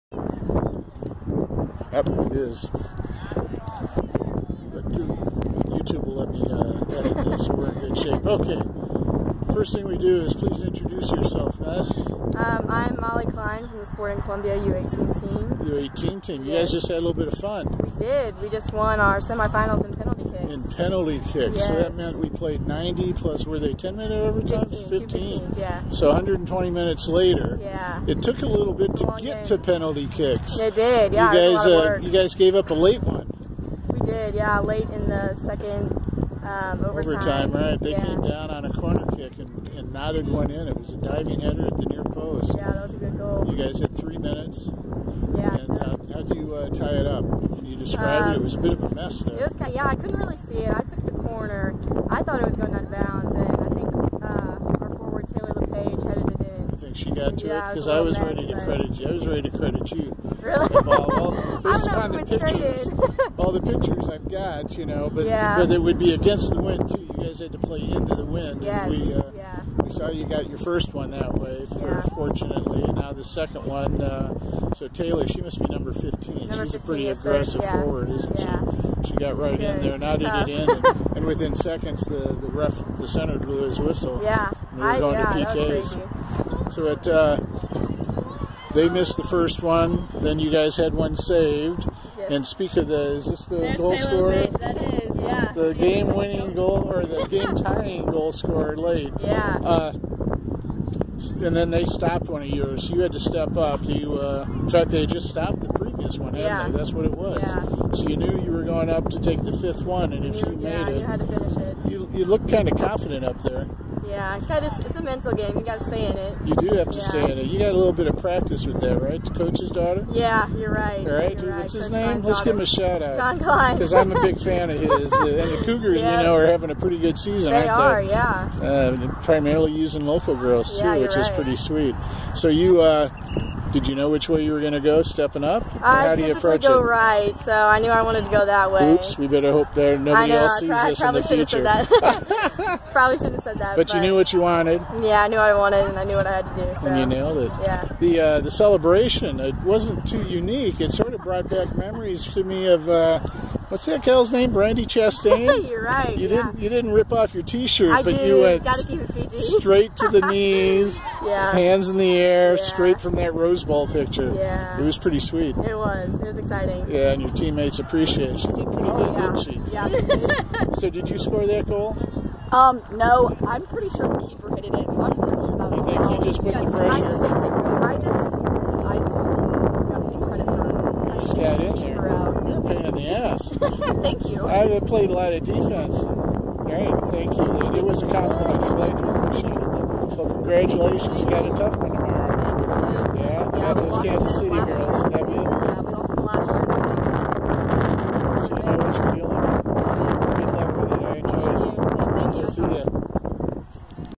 Molly Klein, Sporting Columbia Navy 97-98 Girls
After scoring the decisive PK in a shootout, we talked to the Rock Bridge midfielder about the match.